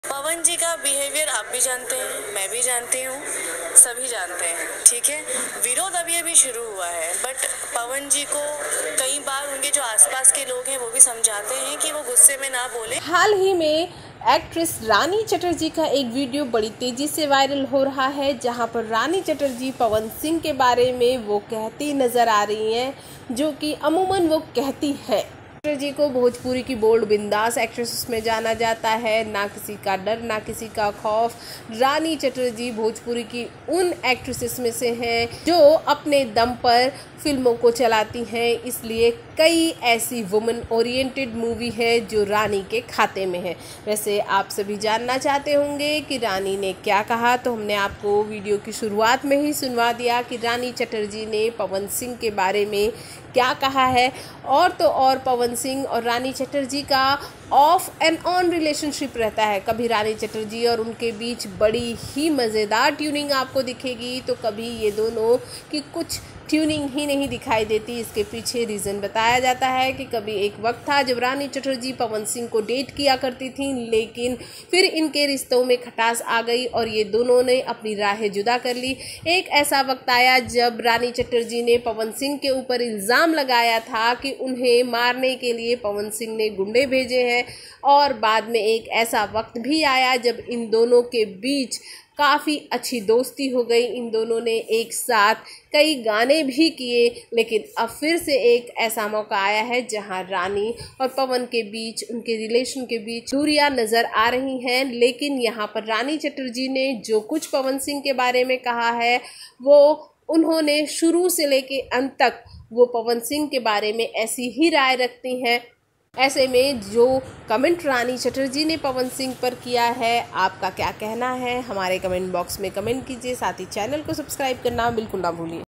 Rani chatterjee ने पवन सिंह को लेकर दिया सबसे विवादित बयान
Ranichatterjee ने पवनसिंह को लेकर दिया सबसे विवादित बयान